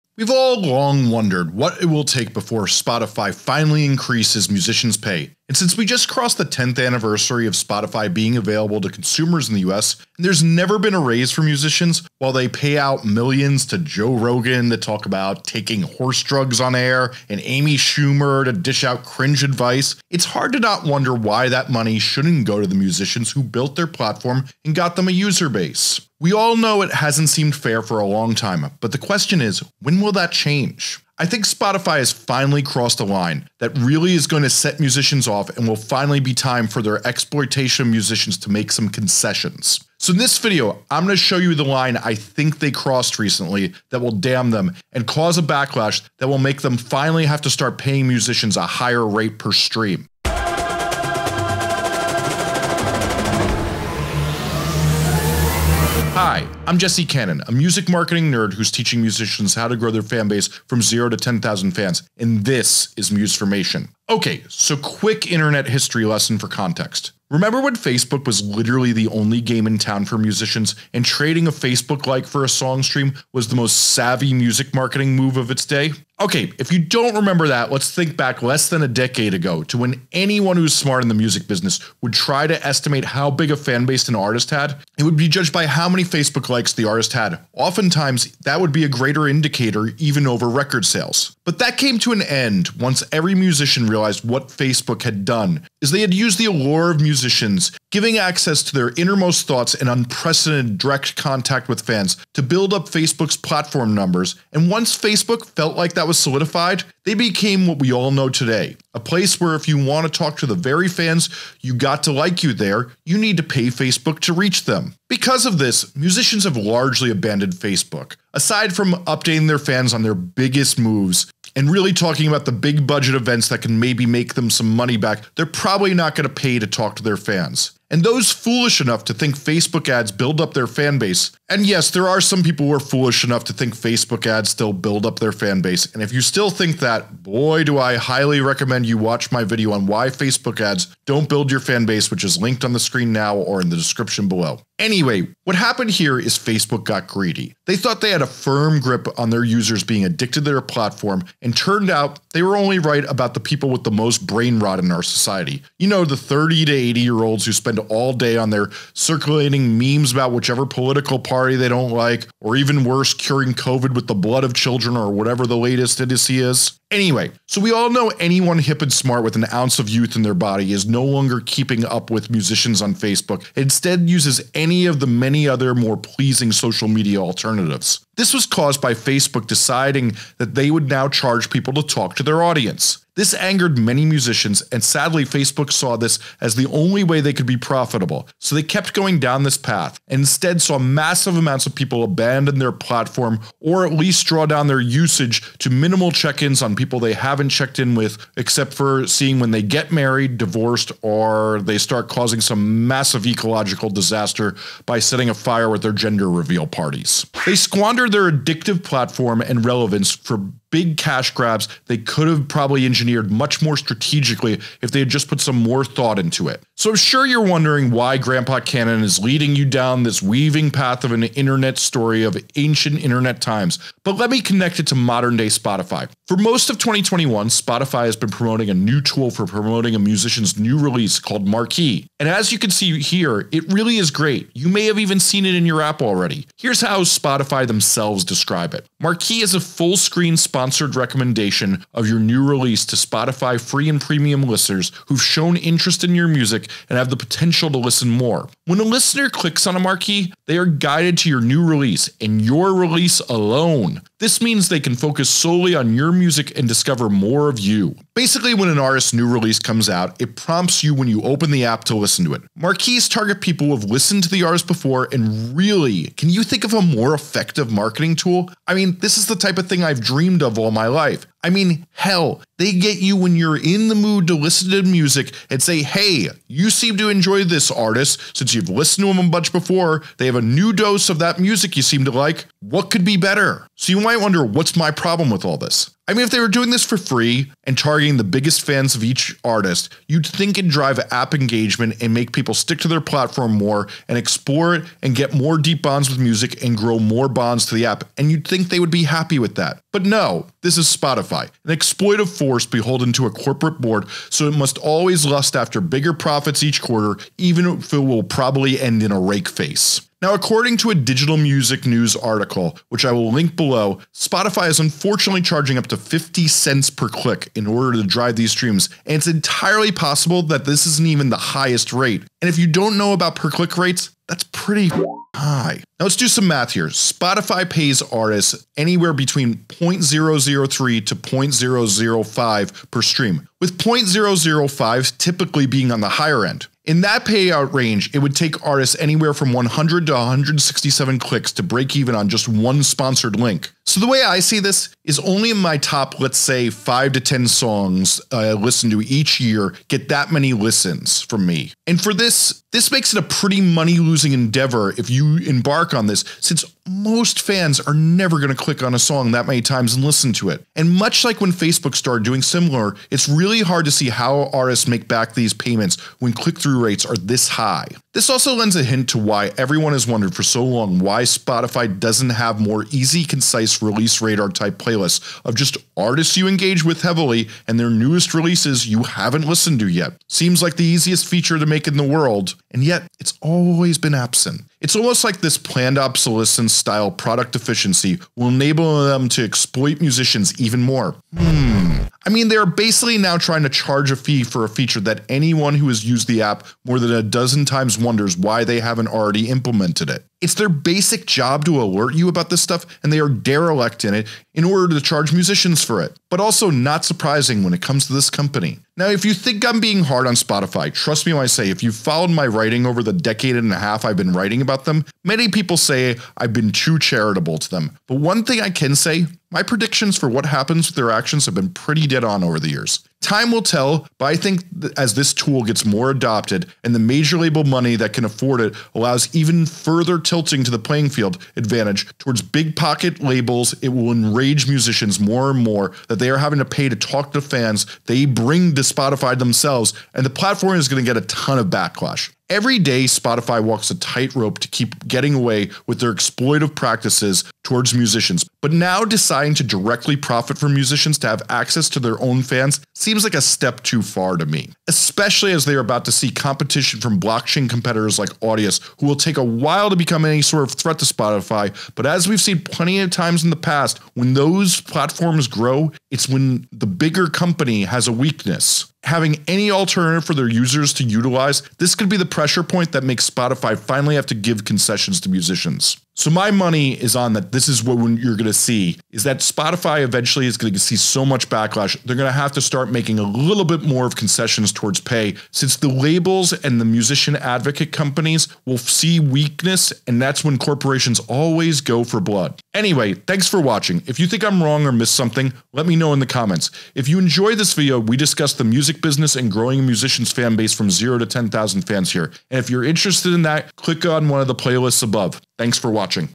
Spotify Just Made A Huge Mistake They Will Pay For
In this video, I discuss how Spotify crossed the line and will inspire a backlash that will finally make them pay musicians more. I break down:
► The line Spotify has crossed that will earn them a huge backlash from the music community
► Why Spotify Marquee feature is an amazing marketing tool but is exploitive of musicians.
► How Spotify’s backlash mirrors when Facebook crossed a similar line in exploiting musicians.

Prepare to unravel the truth about Spotify's recent actions in this video! We dissect the critical moment when Spotify crossed a line, sparking outrage within the music community. Join the conversation as we delve into why Spotify's Marquee feature, while seemingly a powerful marketing tool, has left musicians feeling exploited. Discover striking parallels between Spotify's actions and Facebook's past missteps in the music industry. It's time to stand up for musicians' rights and explore how this backlash might finally push Spotify to pay artists their due. Tune in and be part of the movement for fair compensation in the music world!